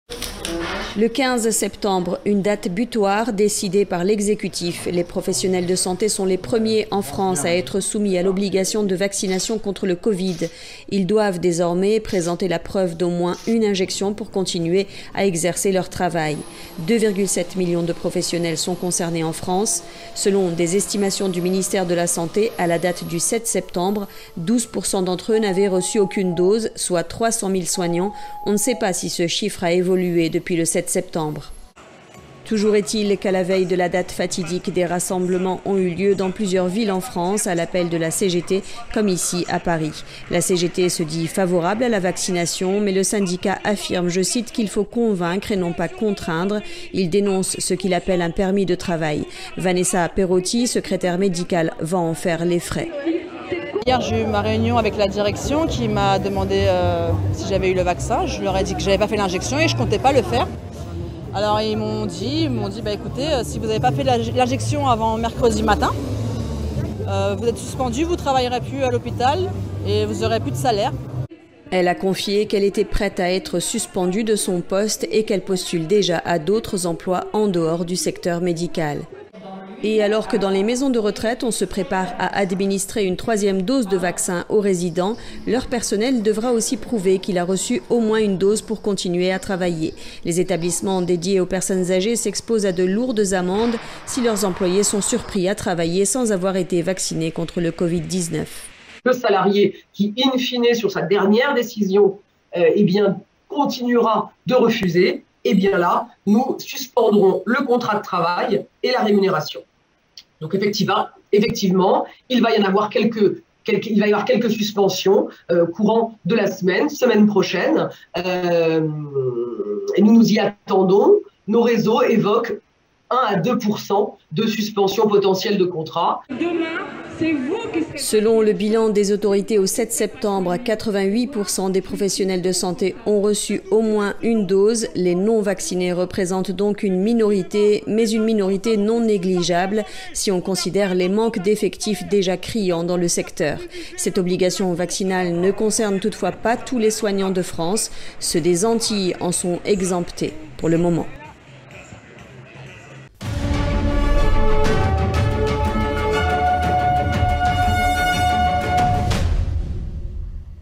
Obligation vaccinale des soignants en vigueur
Le gouvernement l’avait décidé il y a deux mois, le parlement et le sénat l’ont adopté et la date butoir des premières obligations vaccinales est arrivée. Depuis ce matin, les professionnels de la santé doivent prouver qu’ils ont reçu au moins une dose de vaccin contre le COVID pour pouvoir continuer à exercer leur métier.

A quelques jours de la date butoir, le ministère de la santé estimait que 12% des soignants n’avaient pas encore reçu d’injection. Voici un point sur la situation et les répercussions auxquels il faut s’attendre